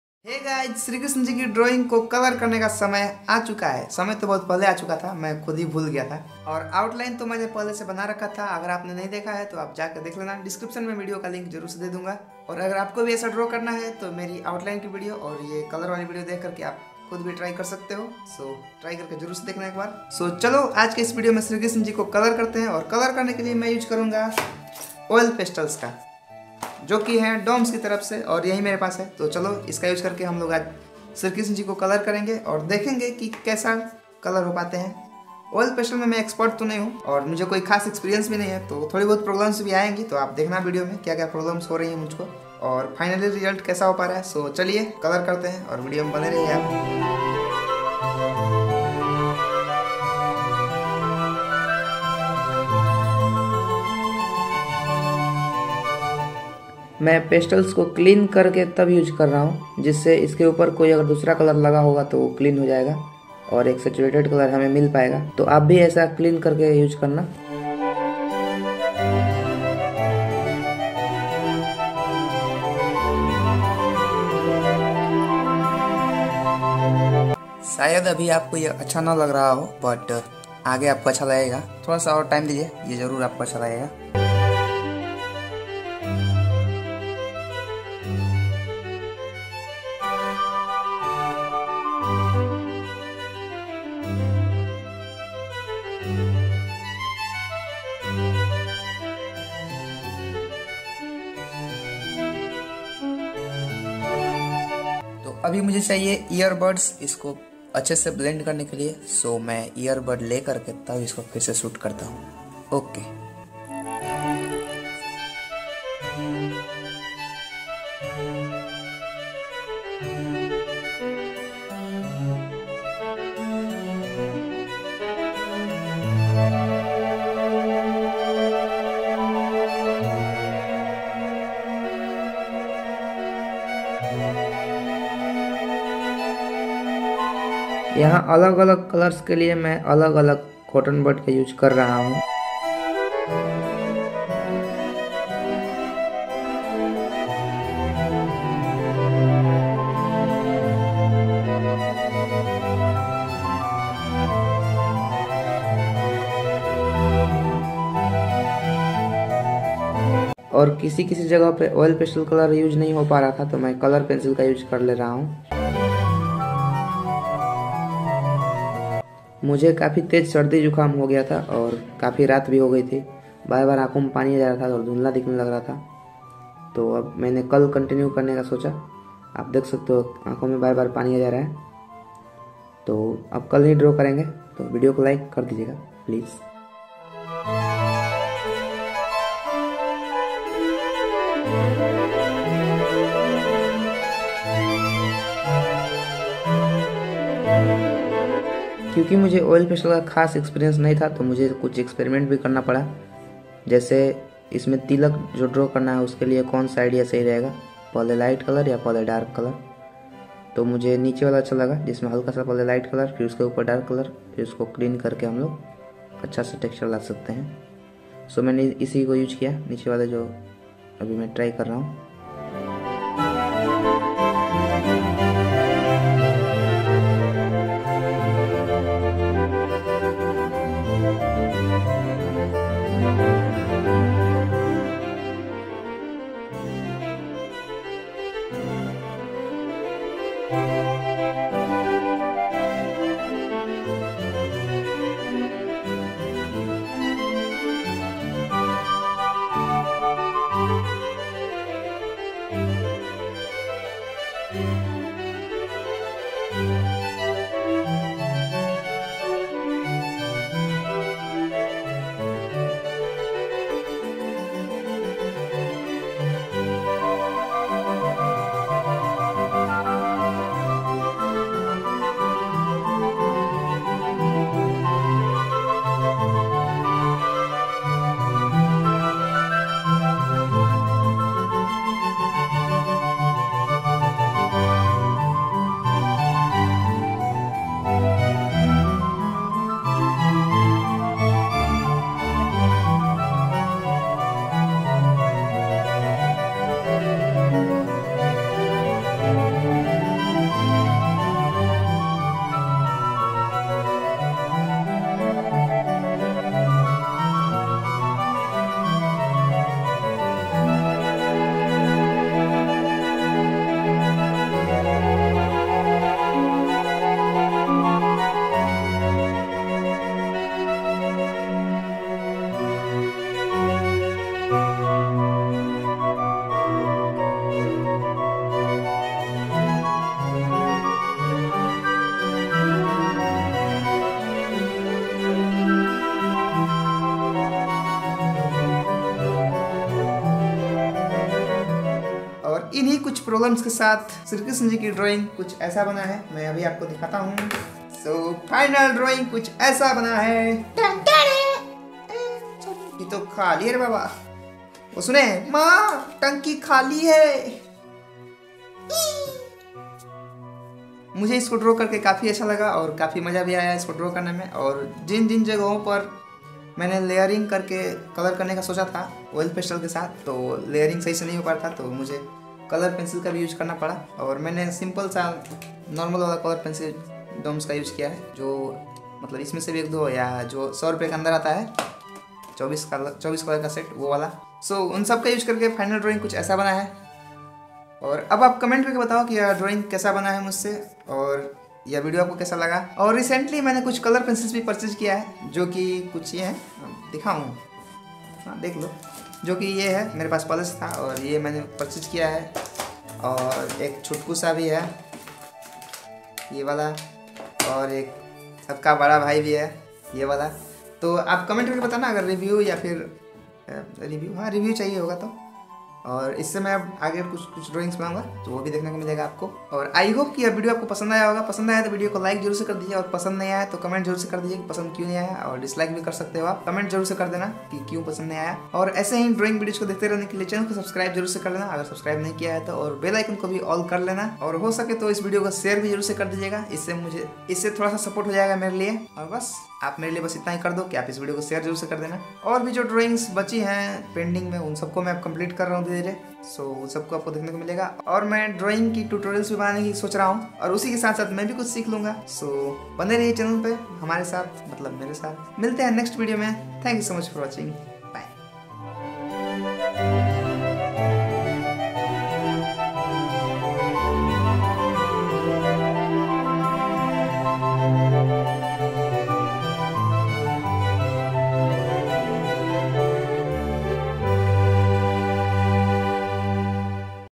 Shree Krishna oil pastel drawing | lord Krishna colour drawing | Sandeep Singh Arts
Lord Krishna drawing and colouring by using Doms  oil pastel colours. Lord Krishna drawing.

I hope you LIKE👍🏻it and COMMENT some words for me🥺
And please Share... to support me 😌

I used normal Doms colour pencils in some areas.

Music from yt music library ________________________________________